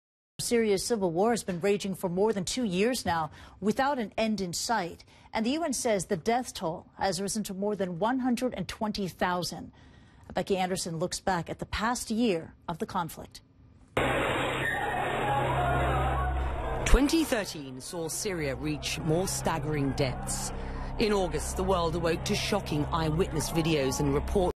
Syria in review CNN com Video
Syria's civil war has been raging for more than two years, with over 100,000 killed. CNN's Becky Anderson reports